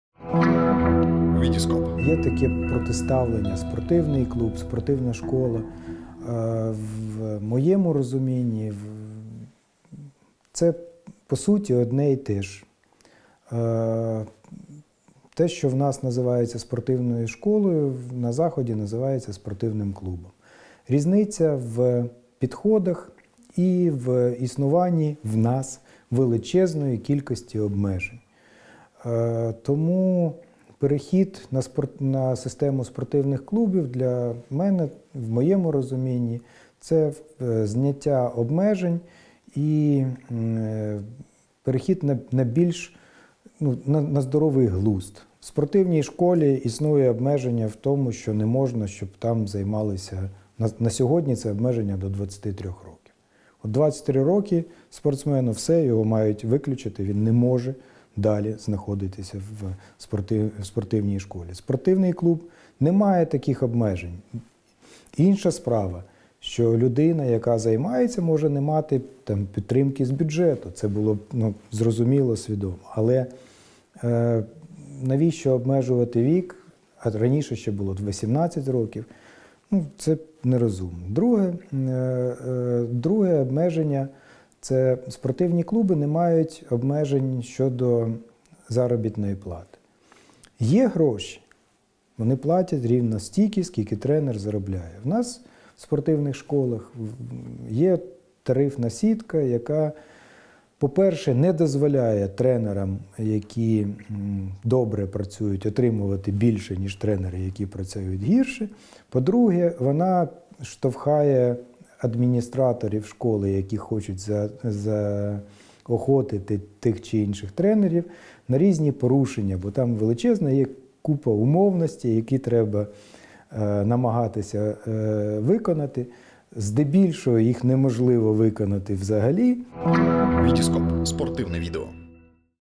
Ігор Гоцул про різницю між спортивною школою і спортивним клубом
Одним із ключових аспектів реформи спортивної галузі має стати перехід на клубну систему. Президент Федерації легкої атлетики України Ігор Гоцул розповідає в чому полягає різниця між спортивною школою і спортивним клубом.

Є таке протиставлення: спортивний клуб і спортивна школа. В моєму розумнні це по суті одне і теж. Те, що у нас називається спортивною школою, на заході називається спортивний клуб. Різниця в підходах й існуванні в нас величезної кількості обмежень. Тому для мене в моєму розумінні перехід на систему спортивних клубів — це зняття обмежень і перехід на здоровий глузд.

В спортивній школі існує обмеження. Не можна, щоб там займалися, на сьогодні це обмеження до 23 років. От у 23 роки спортсмена мають виключити, він далі не може займатися у спортивній школі. У спортивному клубі немає таких обмежень. Інша справа, що людина, яка займається може не мати бюджетної підтримки — це зрозуміло і свідомо, але навіщо обмежувати вік. Це не розумно.

Друге обмеження — це спортивний клуб не має обмежень, щодо заробітної плати, є гроші, вони платять рівно стільки, скільки тренер заробляє. У нас в спортивних школах є тарифна сітка, яка, по-перше, не дозволяє тренерам, які працюють добре, заробляти більше ніж тренери, які працюють гірше. По-друге, вона штовхає адміністрацію школи, яка бажає, щоб тренер заробляв більше, на величезні порушення, бо там є купа умовностей, які треба вирішити. Здебільшого їх неможливо вирішити взагалі.